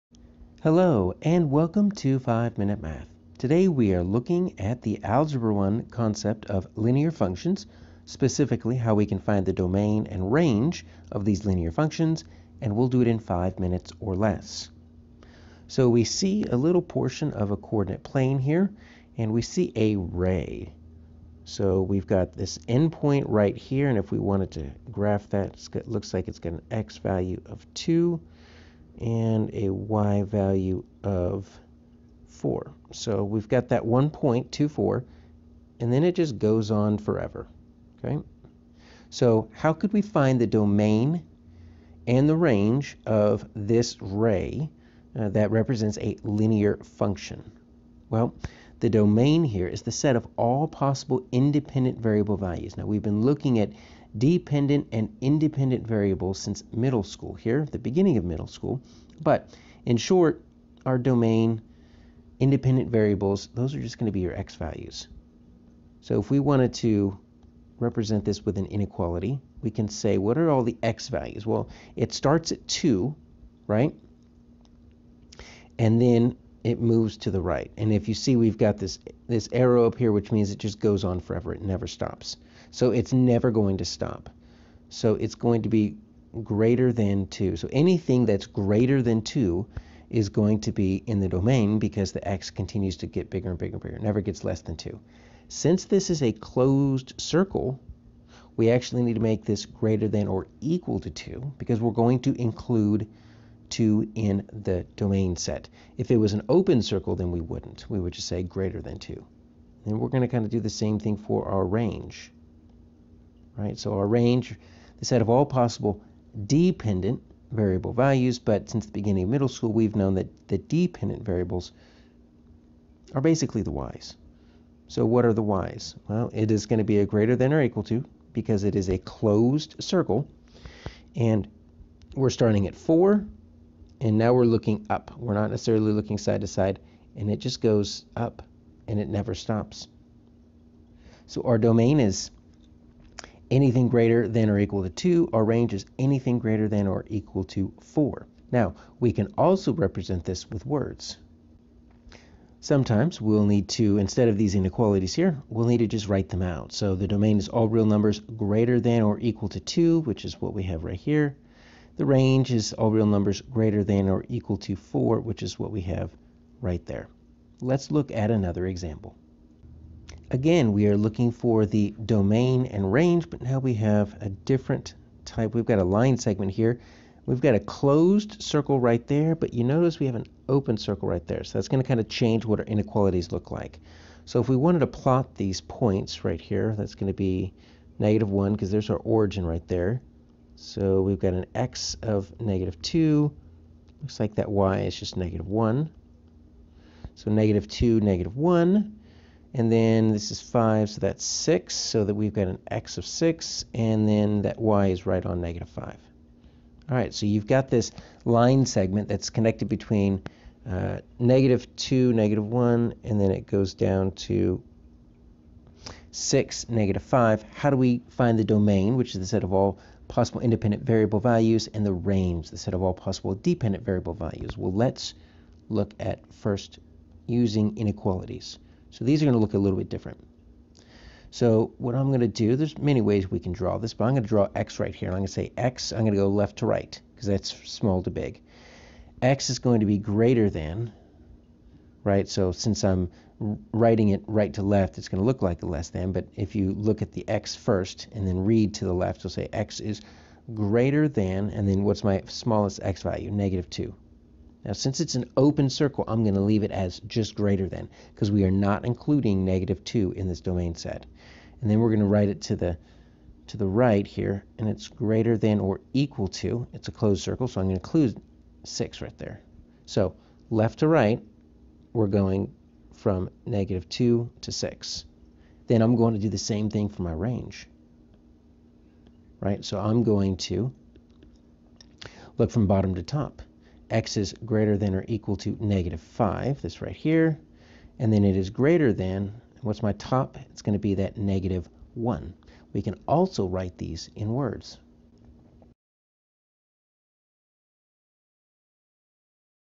Algebra 1 Domain and Range of Linear Functions
Learn how to determine the domain and range of a linear function in mathematical problems; determine reasonable domain and range values for real-world situations, both continuous and discrete; and represent domain and range using inequalities.